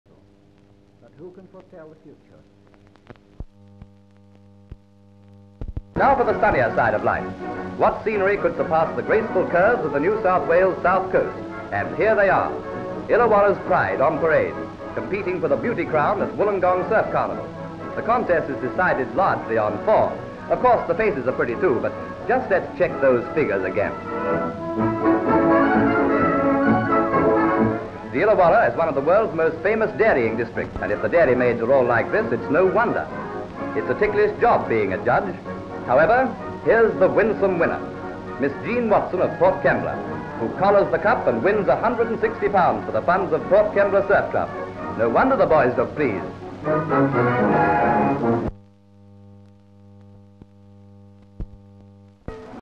BATHING BEAUTIES - SOUND
Bathing Beauties parade in a competition to find "Miss Illawarra" Finally Miss Jean Watson is selected. Past camera - close shots legs fast camera.